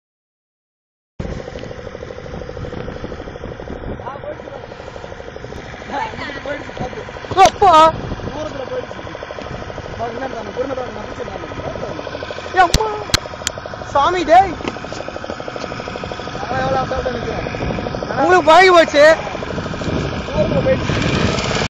Kodiyakkarai - Palk Bay - Boat Ride - SN Sudharsan - 3
This place is where the map of Tamil Nadu, in the Nagapattinam district, where the land takes a literal 90 degree turn. This was shot in 2013.